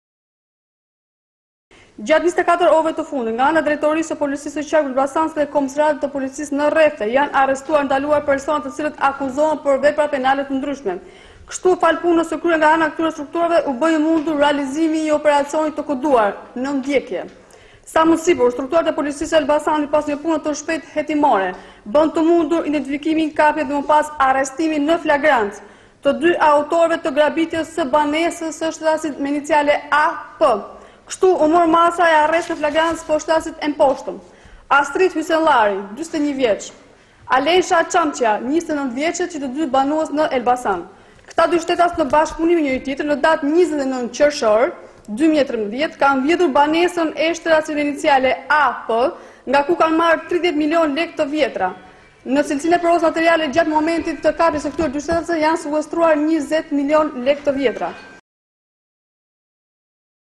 Elbasan, 2 grabitje banesash në 2 ditë, 3 të arrestuar
ELBASAN - Dy grabitje banesash kanë ndodhur në harkun kohorë të dy ditëve në qytetin e Elbasanit, për të cilat policia vuri në pranga 3 persona. Grabitja e parë ndodhi mbrëmjen e 29 qershorit kur kryefamiljari Ardian Peqini kishte shkuar me familjen për të darkuar në periferi të qyteti të Elbasanit. Dy hajdutë kanë hyrë brenda shtëpisë së tij në lagjen "Beqir Dardha" dhe kanë vjedhur rreth 30 milonë lekë të vjetra. Në momentin e parë që ka mbërritur në shtëpi Ardian Peqini, ka parë dritaren e pasme të shtëpisë të shqyer dhe ka njoftuar menjëherë policinë.